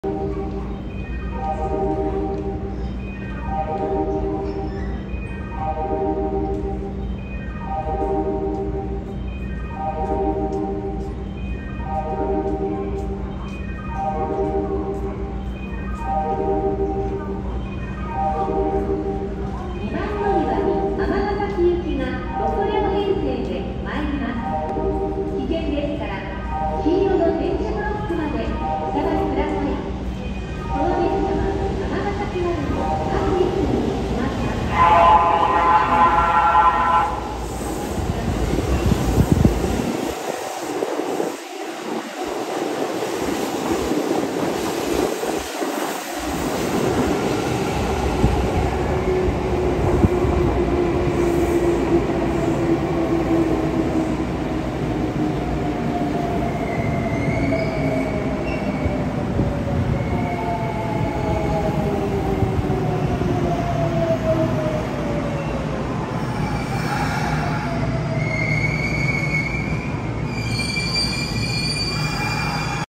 ［近鉄］ しまかぜ サービスホーン ありがとうございます！ ならしかトレイン VH29 との並び
上本町駅にて